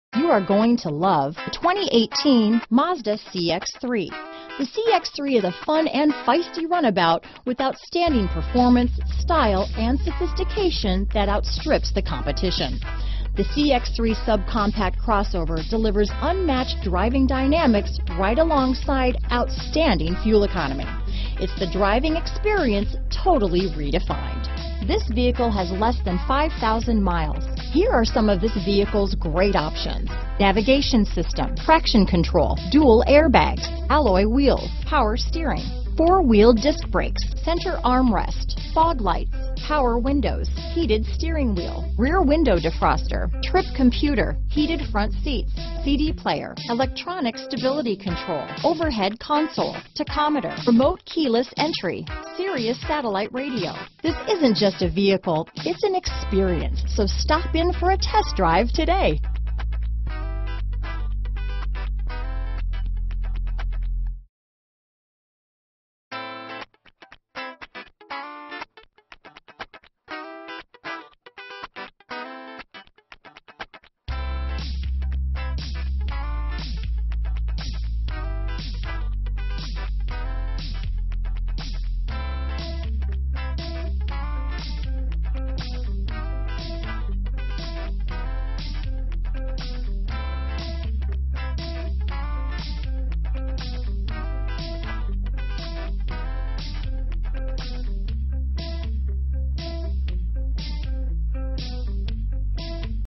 2018 Mazda CX-3 at Oxmoor Mazda | Louisville & Lexington, KY MU5624
Soul Red Metallic Used 2018 Mazda CX-3 available at Oxmoor Mazda in Louisville, Kentucky. Servicing the Lexington, Elizabethtown, KY New Albany, IN Jeffersonville, IN area.
2018 Mazda CX-3 Grand Touring - Stock#: MU5624 - VIN#: JM1DKDD77J0326065

Oxmoor Mazda
7913 Shelbyville Rd